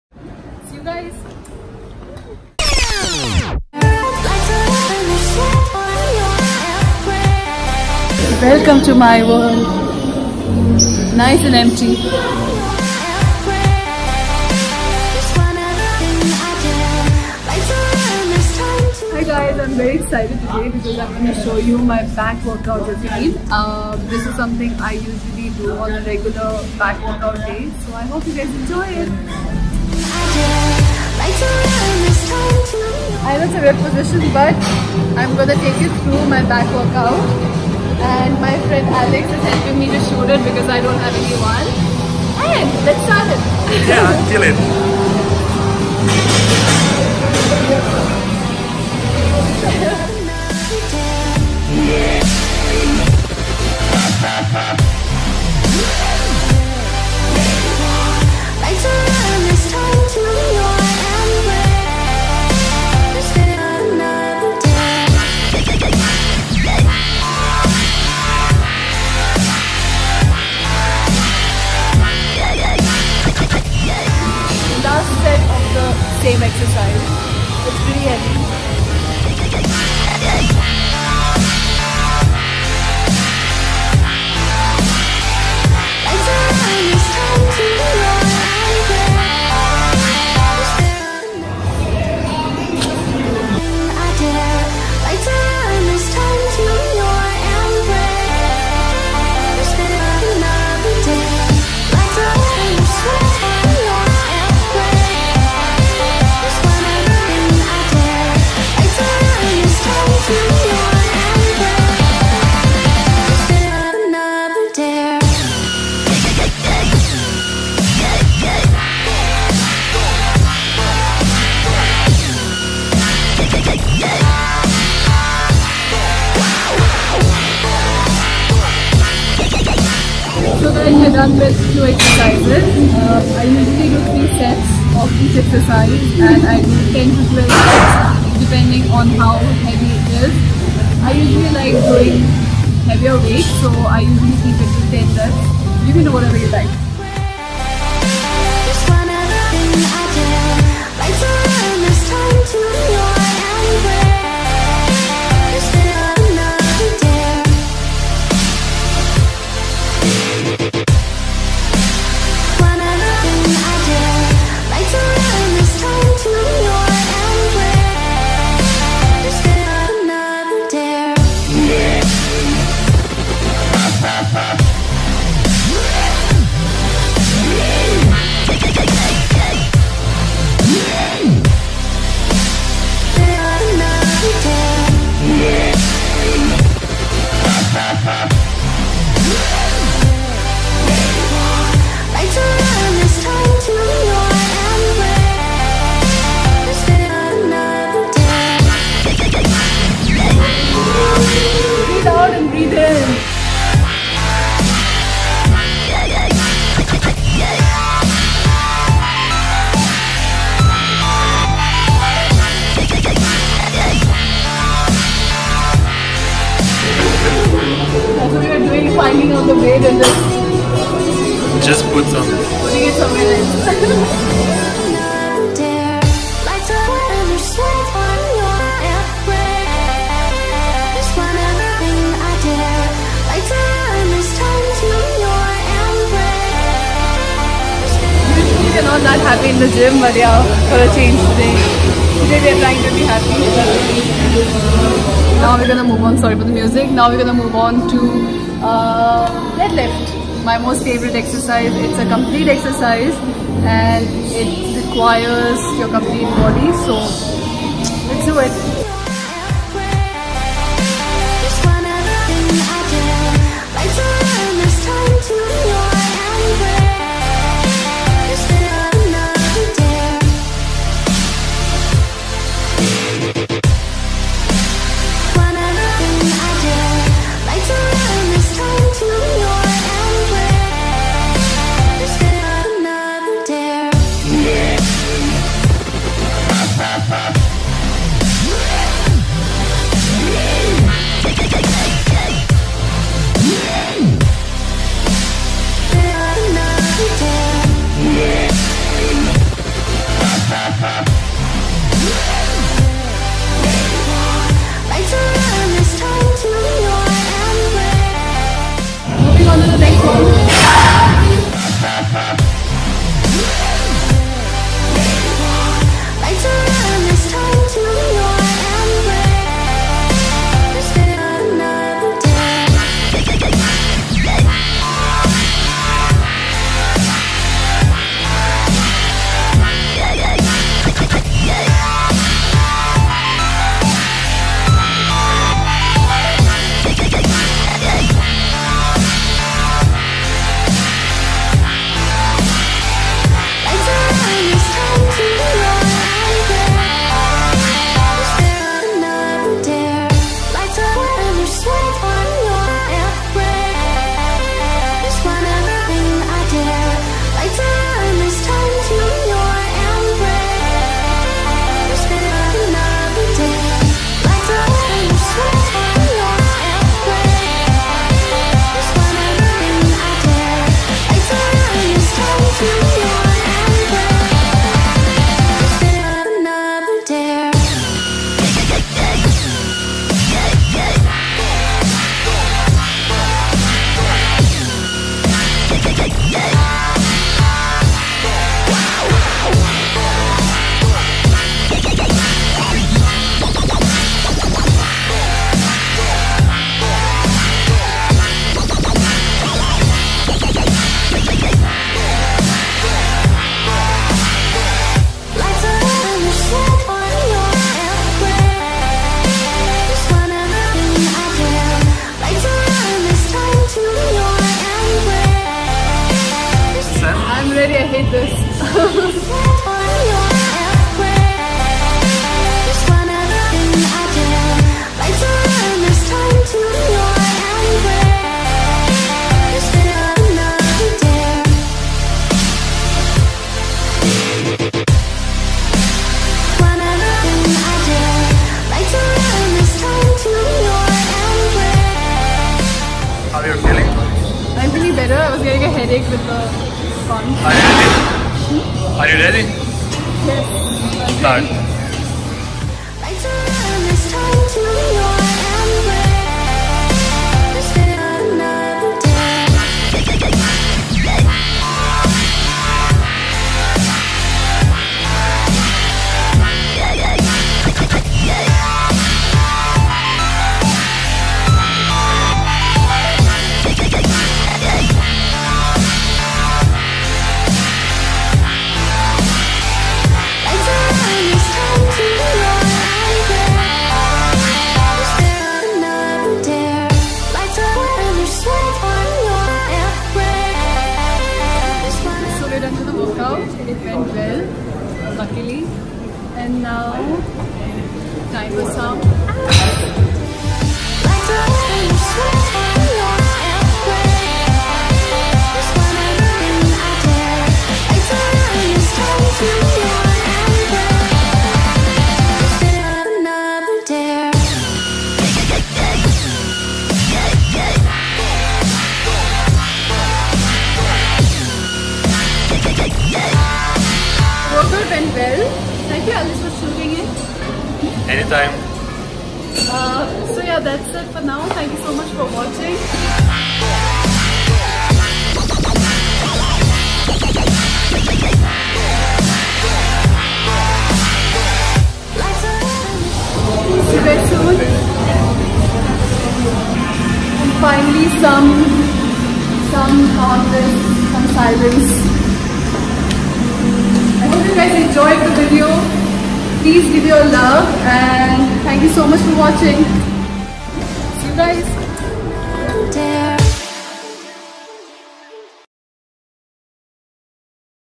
Disha Patani || Back Workout Routine || Really Hot
Disha's 1st workout routine video with all of you 💪This one includes all the exercises that are part of disha's back workout.

►  Our sources
The majority of the content found in this video was used from our Video Blocks & AP Archive Account.